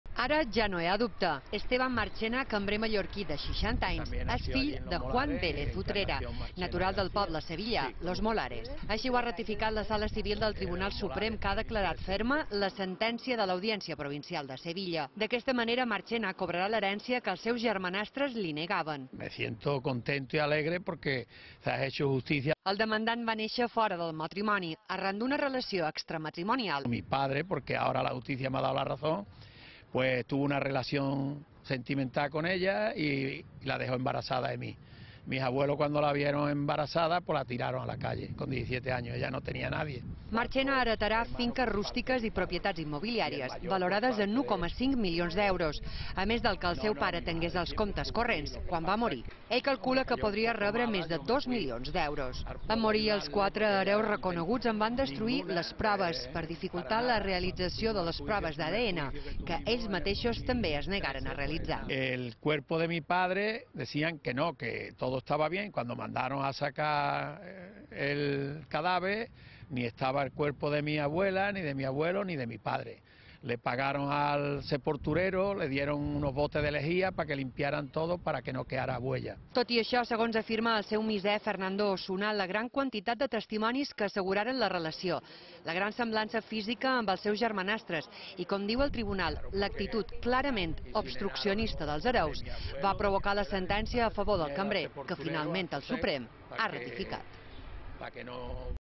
El Suprem ratifica que un cambrer que viu a Mallorca és fill biològic d’un terratinent sevillà
Ara ja no hi ha dubte. Esteban Marchena, un cambrer que viu a Mallorca i té 60 anys, és fill de Juan Vélez Utrera, natural del poble sevillà Los Molares.  Així ho ha ratificat la sala Civil del Tribunal Suprem, que ha declarat ferma la sentència de l’Audiència Provincial de Sevilla. D’aquesta manera Marchena cobrarà l’herència que els seus germanastres li negaven. El demandant va néixer fora del matrimoni, arran d’una relació extramatrimonial entre el seu pare i la seva mare que feia de minyona a la casa.

Marchena heretarà finques rústiques i propietats immobiliàries valorades en 1,5 milions d’euros, a més, del que el seu pare tengués als comptes corrents quan va morir. Ell calcula que podria rebre més de 2 milions d’euros. En morir, els quatre hereus reconeguts en van destruir les proves, per dificultar la realització de les proves d’ADN que ells mateixos també es negaren a realitzar.

Tot i això, segons afirma el seu misser Fernando Osuna la gran quantitat de testimonis que asseguraren la relació, la gran semblança física amb els seus germanastres i com diu el tribunal l’actitud “clarament obstruccionista” dels hereus va provocar la sentència a favor del cambrer que, finalment, el Suprem ha ratificat